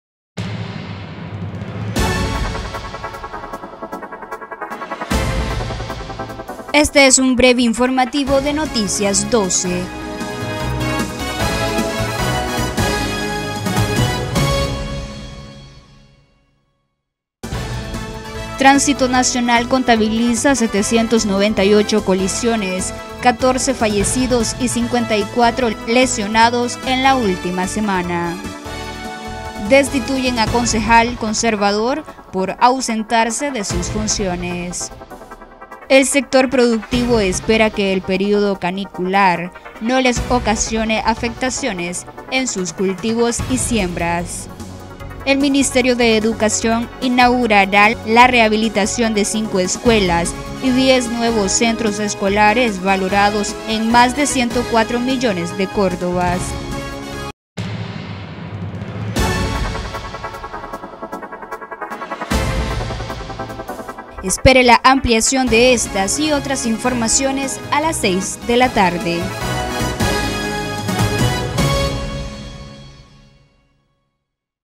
Breve Informativo
📍Tránsito contabiliza 798 accidentes y 14 fallecidos en la última semana
📍Destituyen a concejal conservador por ausentarse de sus funciones
📍Productores espera que canícula no afecte los cultivos
📍MINE inaugurará 10 nuevos centros escolares